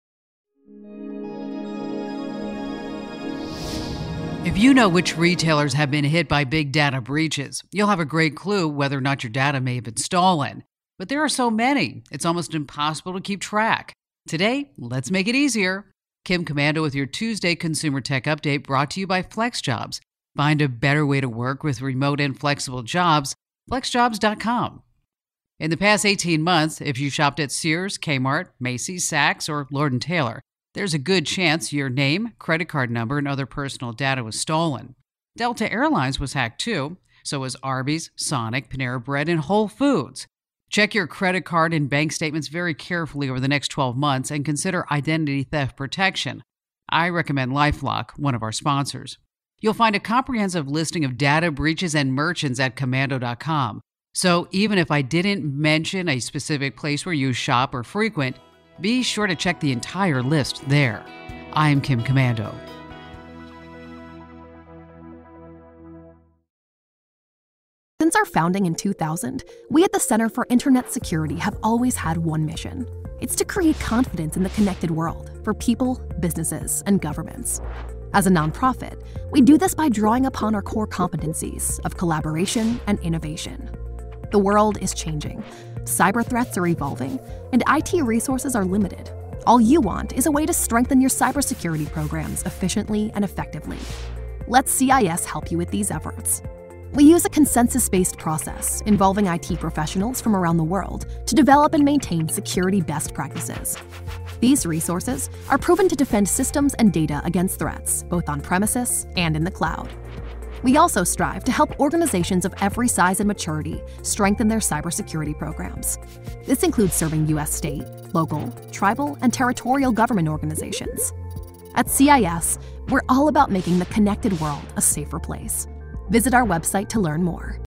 Watch out for retailers where your personal data is likely to be stolen
Nowadays we hear of retailers getting hit by data breaches on a regular basis. But how do we keep track of which ones, and whether or not your data is at risk? Kim Komando has made it easy. Here are the top retailers that likely had been hit. Check your credit cards!